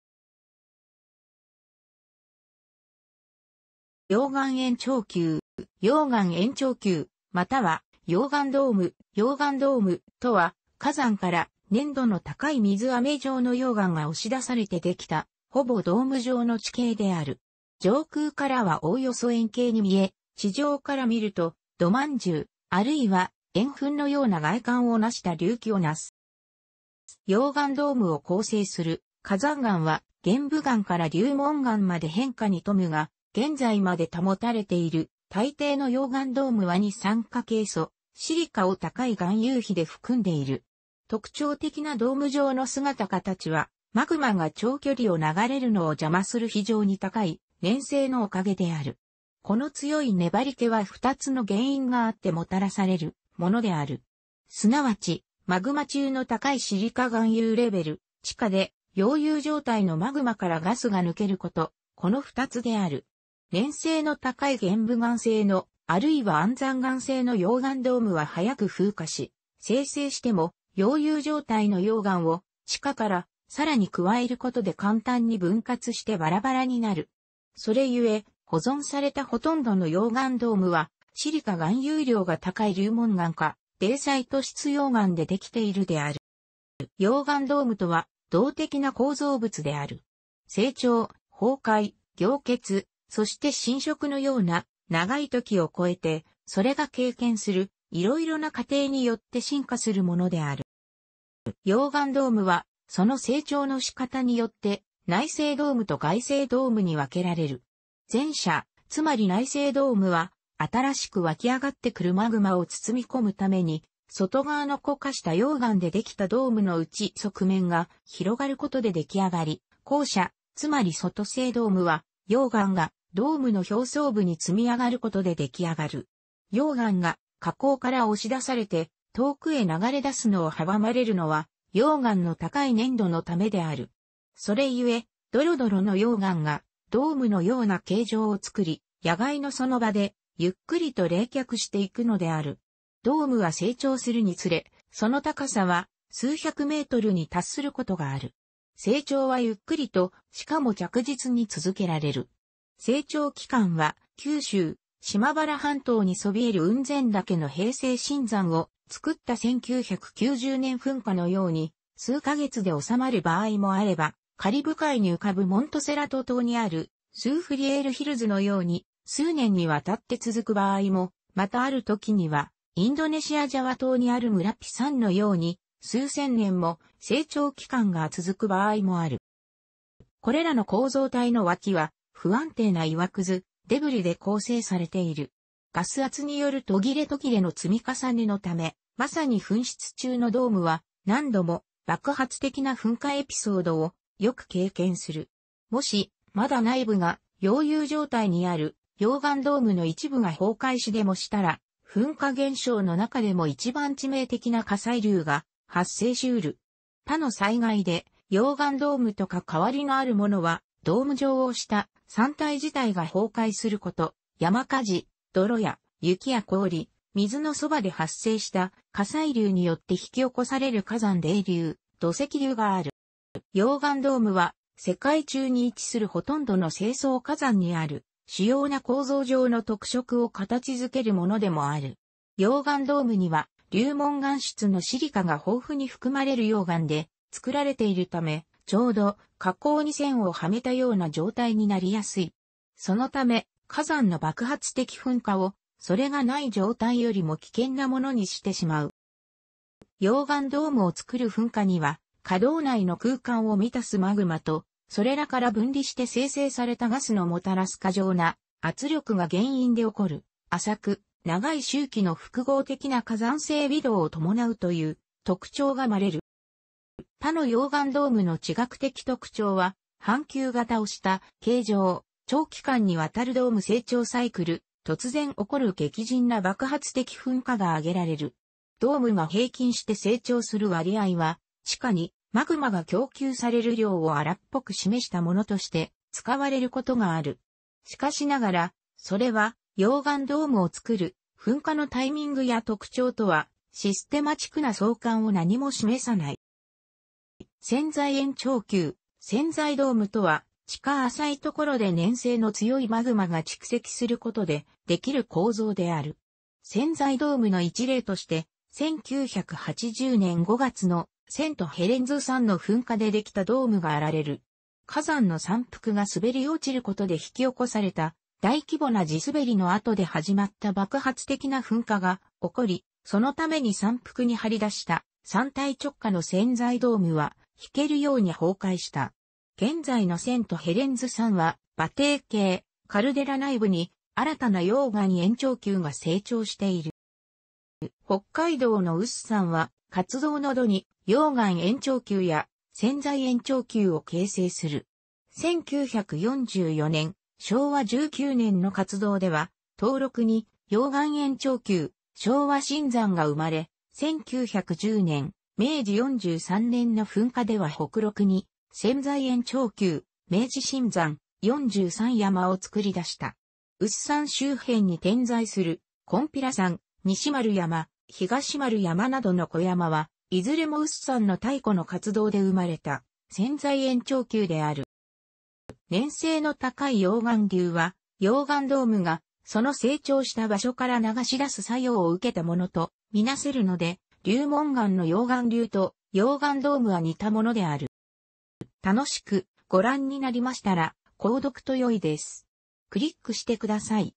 溶岩円頂丘
溶岩円頂丘（ようがんえんちょうきゅう）または溶岩ドーム（ようがんドーム、）とは、火山から粘度の高い水飴状の溶岩が押し出されてできた、ほぼドーム状の地形である。上空からはおおよそ円形に見え、地上から見ると土饅頭、あるいは円墳のような外観を成した隆起を成す。

溶岩ドームを構成する火山岩は、玄武岩から流紋岩まで変化に富むが、現在まで保たれている大抵の溶岩ドームは二酸化ケイ素（シリカ）を高い含有比で含んでいる。特徴的なドーム状の姿形は、マグマが長距離を流れるのを邪魔する非常に高い粘性のおかげである。この強い粘り気は二つの原因が有ってもたらされるものである。即ち、マグマ中の高いシリカ含有レベル、地下で溶融状態のマグマからガスが抜けること、この2つである。粘性の高い玄武岩性の、あるいは安山岩性の溶岩ドームは早く風化し、生成しても溶融状態の溶岩を地下からさらに加えることで簡単に分割してばらばらになる。それゆえ、保存された殆どの溶岩ドームはシリカ含有量が高い流紋岩かデイサイト質溶岩でできているのである。

溶岩ドームとは、動的な構造物である。成長、崩壊、凝結、そして侵食のような、長い時を超えて、それが経験するいろいろな過程によって進化するものである。

溶岩ドームは、その成長の仕方によって、内成（ないせい）ドームと外成（がいせい）ドームに分けられる。前者、つまり内成ドームは、新しく湧き上がってくるマグマを包み込むために、外側の固化した溶岩でできたドームの内側面が拡がることででき上がり、後者、つまり外成ドームは、溶岩がドームの表層部に積みあがることででき上がる。溶岩が火口から押し出されて遠くへ流れ出すのを阻まれるのは、溶岩の高い粘度のためである。それゆえ、どろどろの溶岩がドームの様な形状をつくり、野外のその場でゆっくりと冷却していくのである。ドームは成長するにつれ、その高さは数百メートルに達することがある。成長はゆっくりと、しかも着実に続けられる。生長期間は、九州・島原半島に聳える雲仙岳の平成新山を作った1990年噴火のように、数か月で収まる場合もあれば、カリブ海に浮かぶモントセラト島にあるスーフリエール・ヒルズのように、数年にわたって続く場合も、またあるときにはインドネシア・ジャワ島にあるムラピ山のように、数千年も生長期間が続く場合もある。

これらの構造体の脇は、不安定な岩屑（デブリ）で構成されている。ガス圧による途切れ途切れの積み重ねのため、まさに噴出中のドームは、何度も爆発的な噴火エピソードをよく経験する。もし、まだ内部が溶融状態にある溶岩ドームの一部が崩壊しでもしたら、噴火現象のなかでも一番致命的な火砕流が発生しうる。他の災害で溶岩ドームとかかわりのあるものは、ドーム状をした山体自体が崩壊すること、山火事、泥や雪や氷、水のそばで発生した火砕流によって引き起こされる火山泥流（土石流）がある。

溶岩ドームは世界中に位置する殆どの成層火山にある、主要な構造上の特色を形付けるものでもある。溶岩ドームには流紋岩質のシリカが豊富に含まれる溶岩で作られているため、丁度、火口に栓を嵌めたような状態になりやすい。そのため、火山の爆発的噴火を、それがない状態よりも危険なものにしてしまう。

溶岩ドームを作る噴火には、火道内の空間を充たすマグマと、それらから分離して生成されたガスのもたらす過剰な圧力が原因で起こる、浅く、長い周期の複合的な火山性微動を伴うという特徴が含まれる。

他の溶岩ドームの地学的特徴は、半球形をした形状、長期間にわたるドーム成長サイクル、突然起こる激甚な爆発的噴火があげられる。ドームが平均して成長する割合は、地下にマグマが供給される量を荒っぽく示したものとして使われることがある。しかしながら、それは溶岩ドームを作る噴火のタイミングや特徴とはシステマチックな相関を何も示さない。

潜在円頂丘（潜在ドーム）とは、地下浅いところで粘性の強いマグマが蓄積することでできる構造である。潜在ドームの一例として、1980年5月のセント・ヘレンズ山の噴火でできたドームがあげられる。火山の山腹が滑り落ちることで引き起こされた大規模な地滑りの後で始まった爆発的な噴火が起こり、そのために山腹に張り出した山体直下の潜在ドームは、弾ける様に崩壊した。現在のセント・ヘレンズ山は、馬蹄形カルデラ内部に新たな溶岩円頂丘が成長している。

北海道の有珠山は、活動の度に溶岩円頂丘や潜在円頂丘を形成する。1944年（昭和19年）の活動では、東麓に溶岩円頂丘・昭和新山が生まれ、...